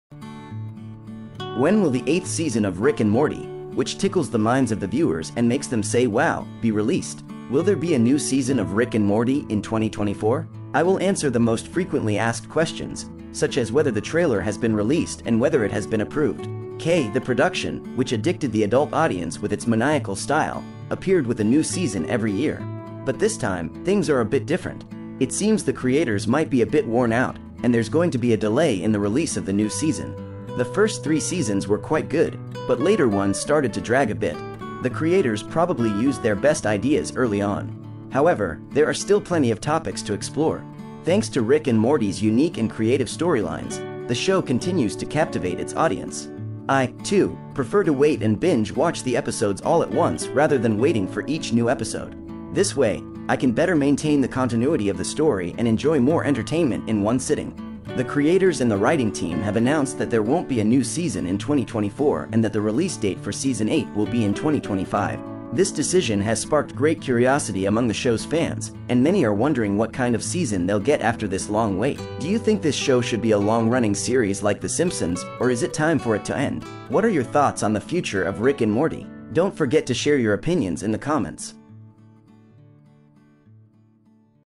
Rick And Morty Season 8: Release Date & What To Expect! Should Season 9 Be The Last?
Rick And Morty Season 8 Release Date!!

When will the 8th season of Rick and Morty, which viewers are eagerly waiting for, be released? Will there be a new season in 2024? Has the trailer been released and has it been approved? In this video, I tried to answer the questions that Rick and Morty fans are most curious about.

To support me; I would appreciate if you could help with your Youtube "Join Button" message: